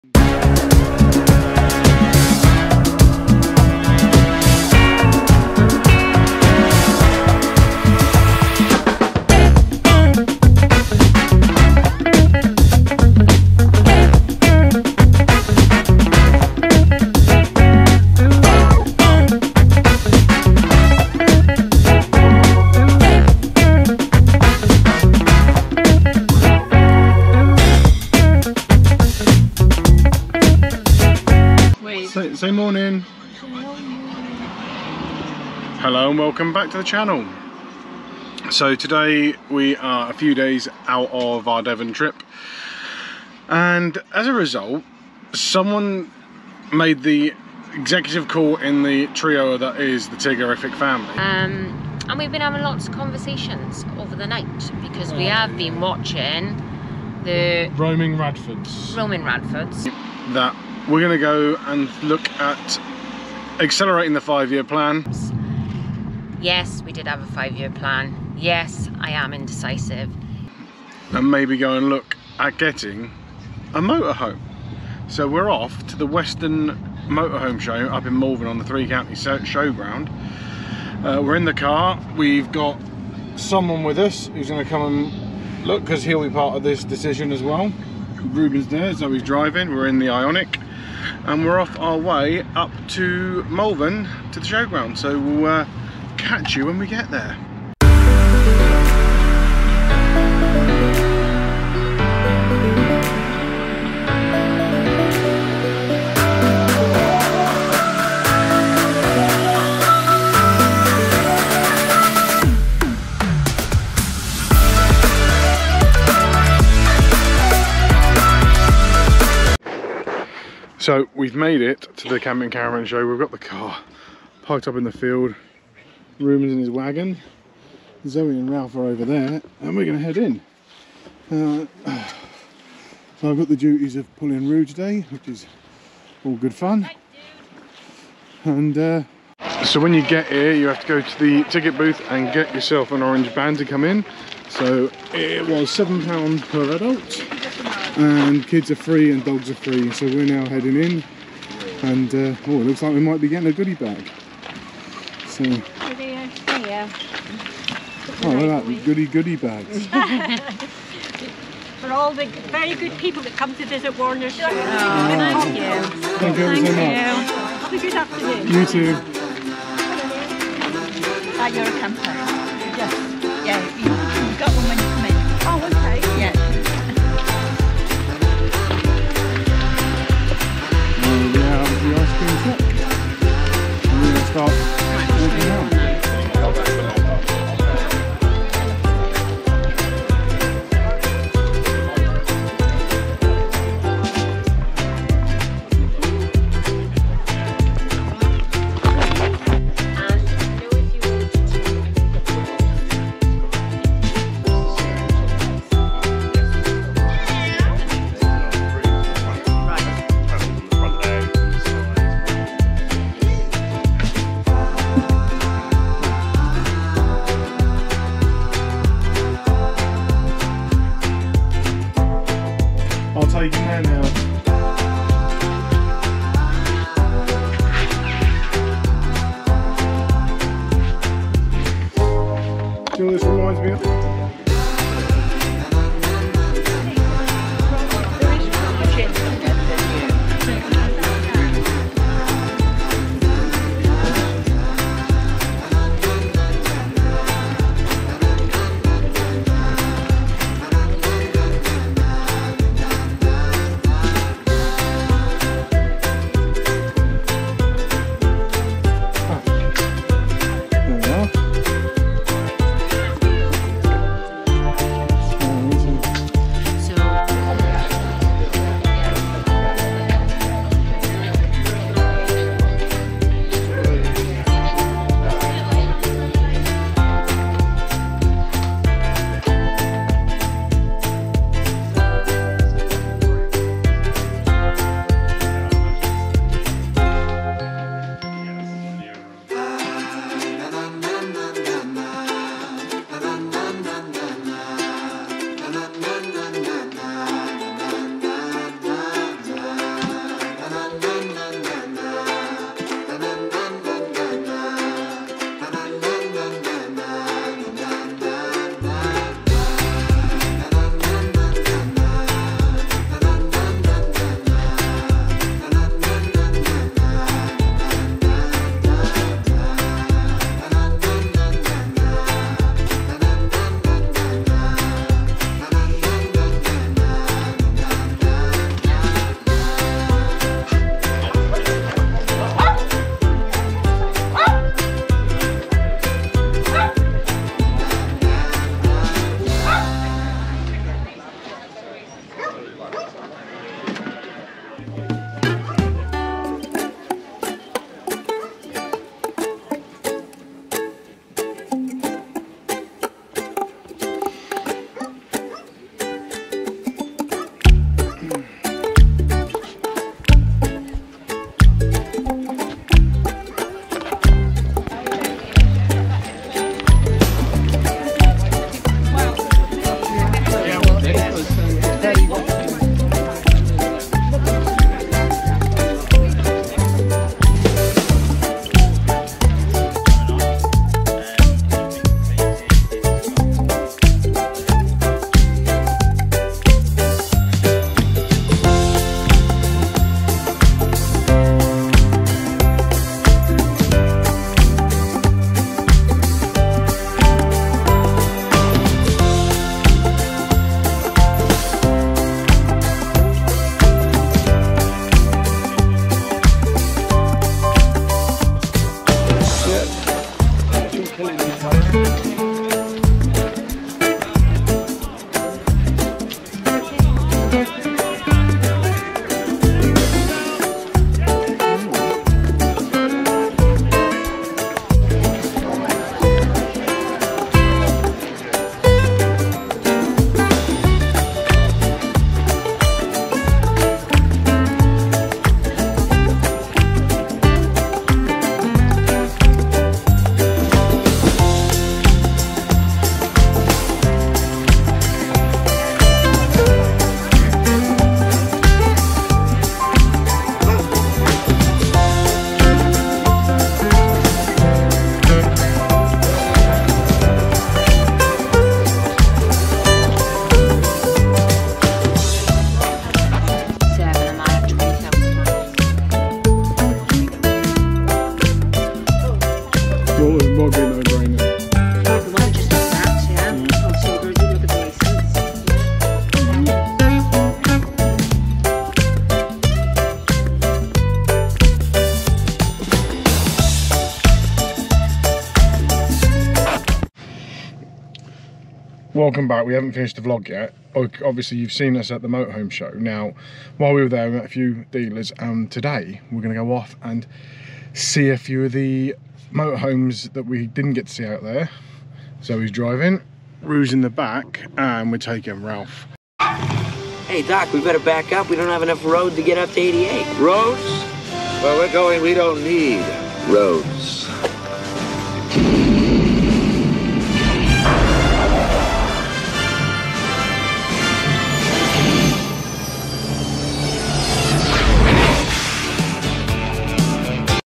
Western Caravan and Motorhome show August - Crap we are Buying a Motorhome ..Maybe wait till end!
Come Join us this week while we go to the 3 County's showground where we visit the Warner Western Caravan and Motorhome show look at some Vans to get an Idea on our next step in the exulcerated 5 year plan ( watch the intro )

Then at the end there maybe a surprise or 2